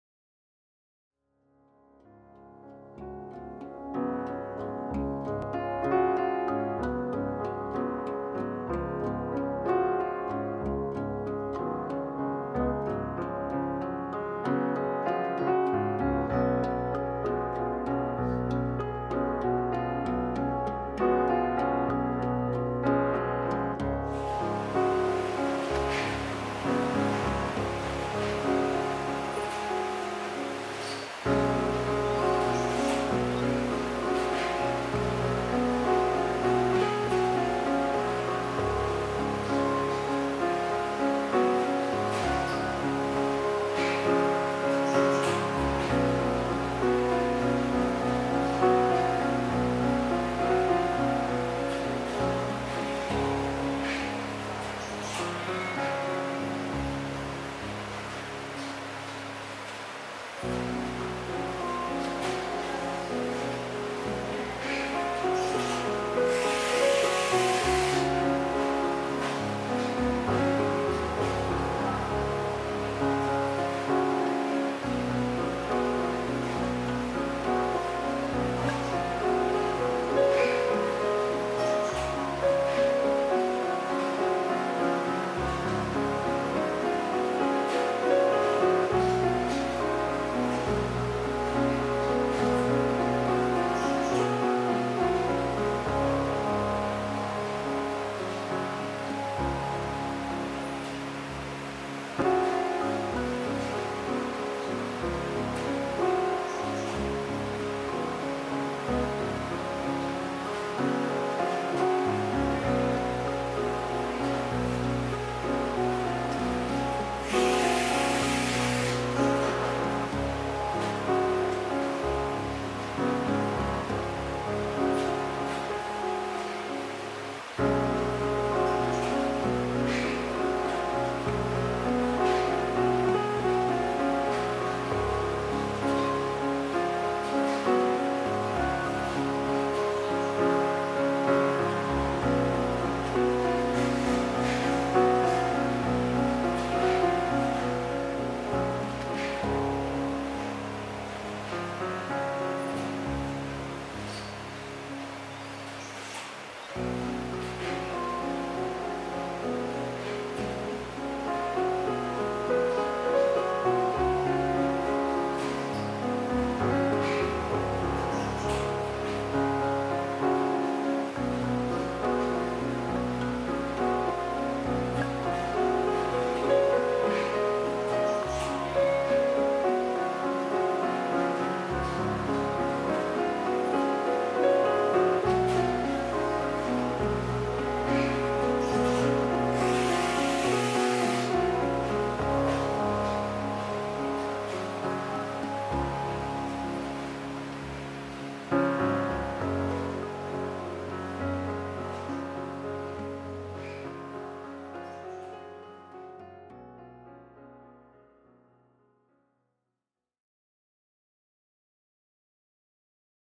Cole's Puddings - Bread Making.
Here at Cole's Puddings we make own bread and its this bread that's used in every pudding we make.